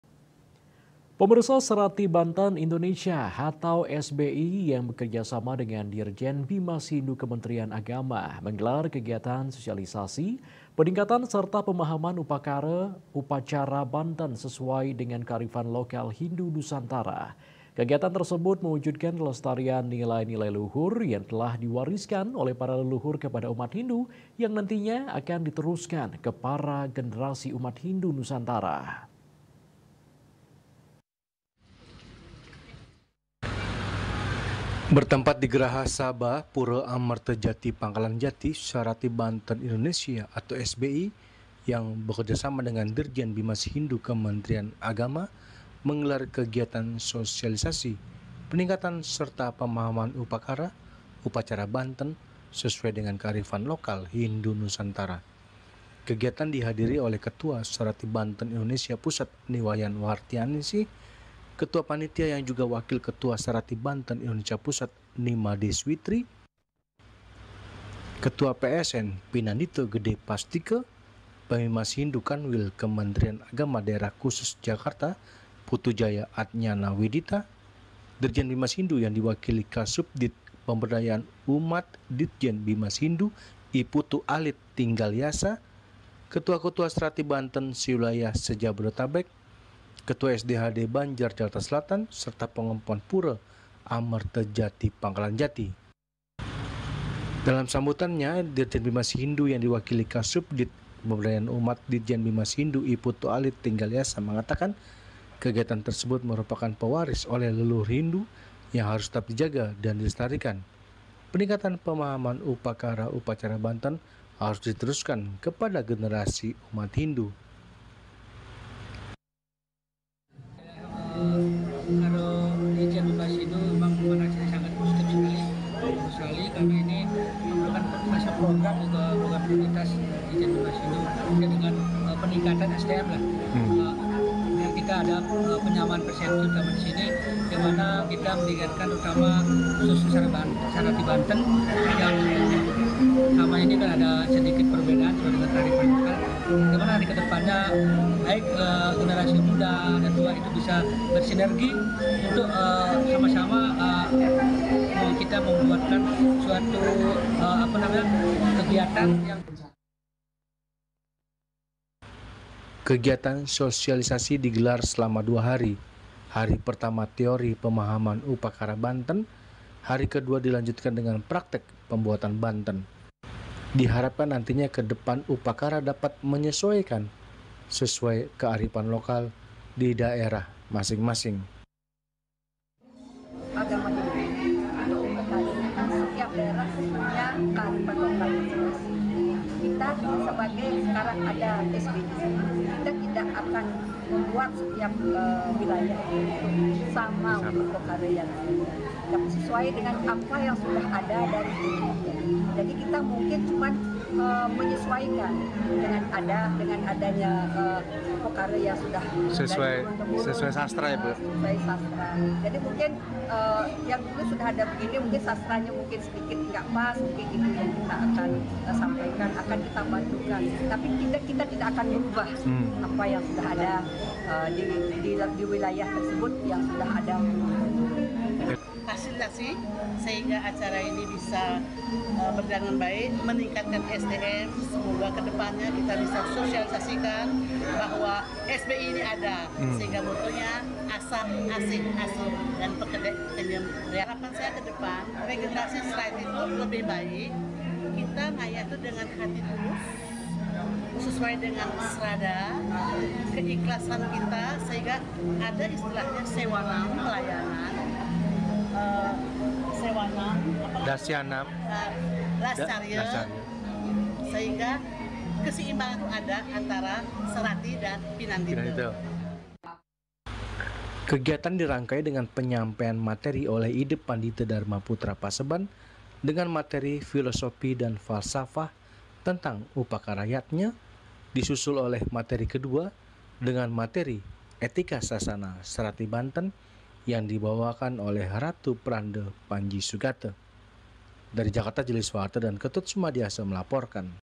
SOSIALISASI KEGIATAN SARATHI BANTEN INDONESIA GUNA PENINGKATAN PEMAHAMAN UPAKARA UPACARA SESUAI KEAR
SARATHI BANTEN INDONESIA ,ATAU SBI ,YANG BEKERJASAMA DENGAN DIRJEN BIMAS HINDU KEMENTERIAN AGAMA, MENGGELAR KEGIATAN SOSIALISASI ,PENINGKATAN SERTA PEMAHAMAN UPAKARA ,UPACARA BANTEN SESUAI DENGAN KEARIFAN LOKAL HINDU NUSANTARA.KEGIATAN TERSEBUT, MEWUJUDKAN KELESTARIAN NILAI NILAI LUHUR, YANG TELAH DI WARISKAN OLEH PARA LELUHUR KEPADA UMAT HINDU, YANG NANTI NYA, AKAN DI TERUSKAN KE PARA GENERASI UMAT HINDU NUSANTARA.